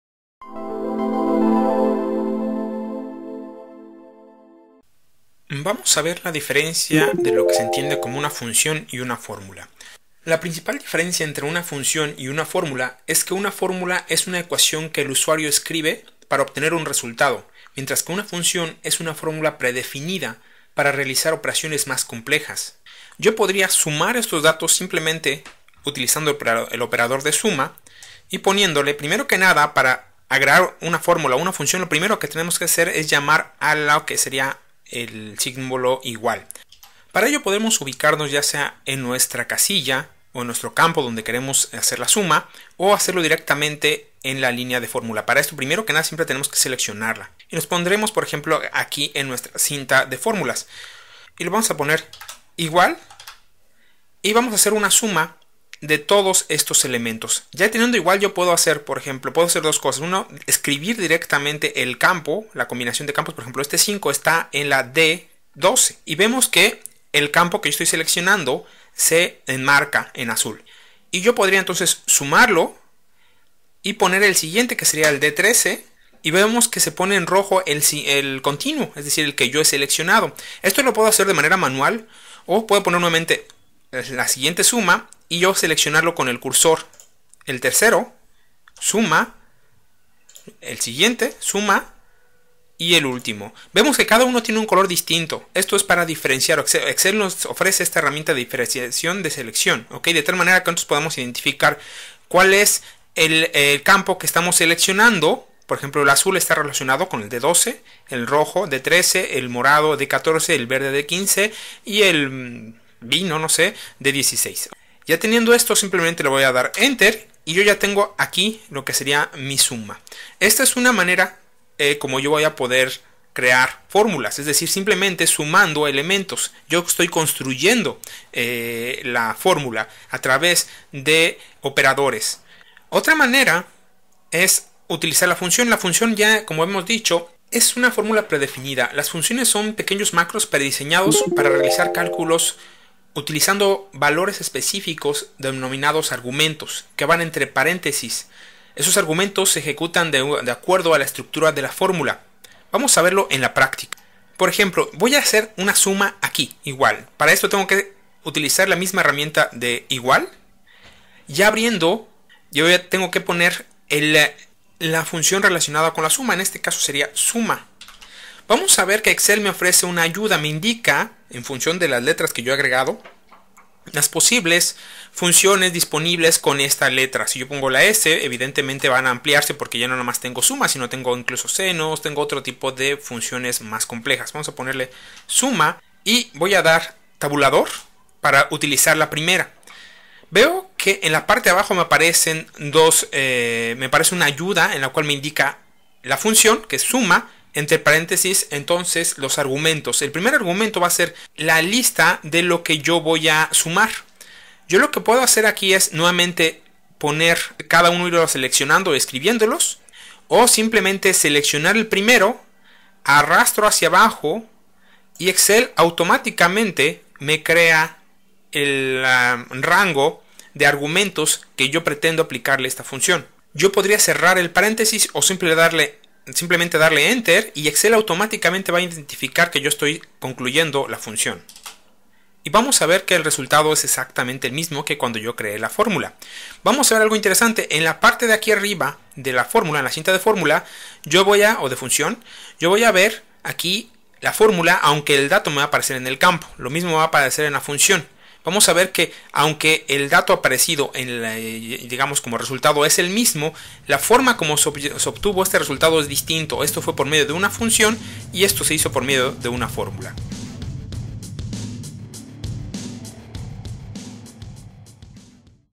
Excel 2016, Diferencia entre función y formula, Curso Fundamentos, cap 4.1
En este tutorial de explico la diferencia entre función y formula, parte del Curso completo de Fundamentos de Microsoft Office Excel 2016 en Windows 10.

¿Quieres ver el curso completo?

¿Quieres encontrar Whygs en otros lugares de Internet?

¿Quieres saber mas sobre cursos completos gratis?